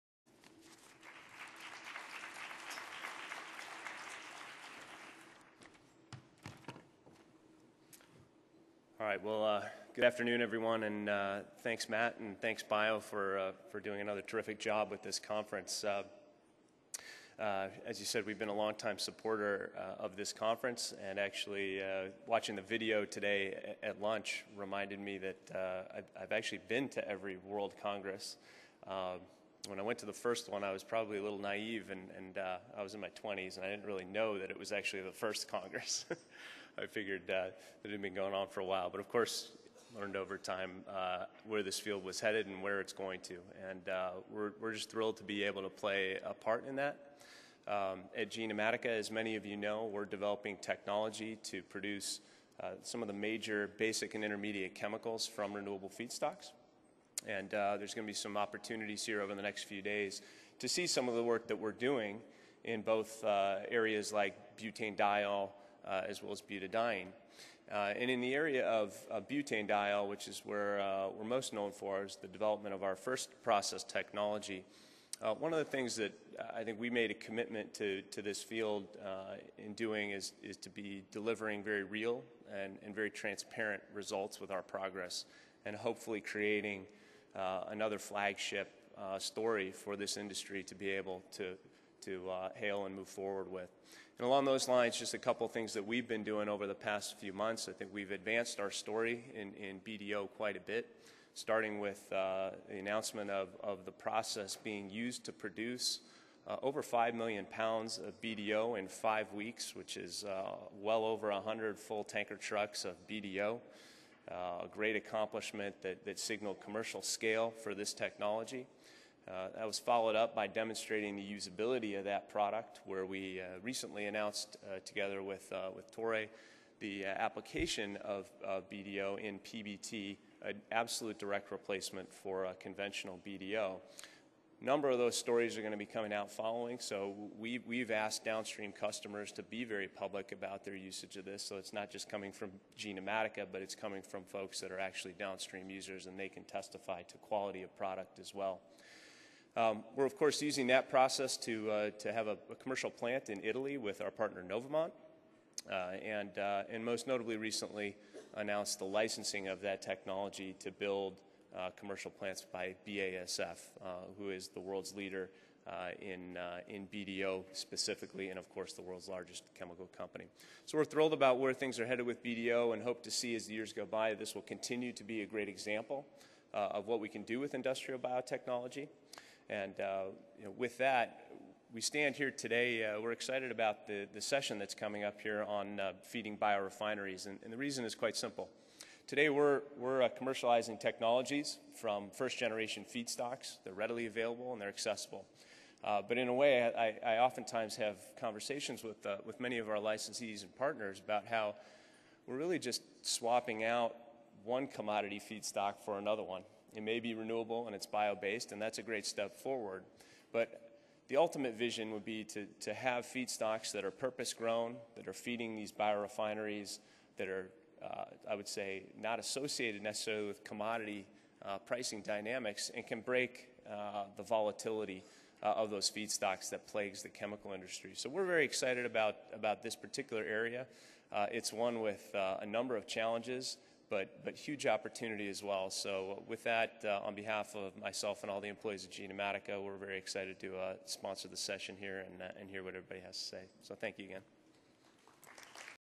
Christophe Schilling of Genomatica Addresses 10th Annual BIO World Congress 2013
Christophe Schilling, Ph.D., Chief Executive Officer of Genomatica addresses attendees of the BIO World Congress on Industrial Biotechnology, on the occasion of the 10th anniversary.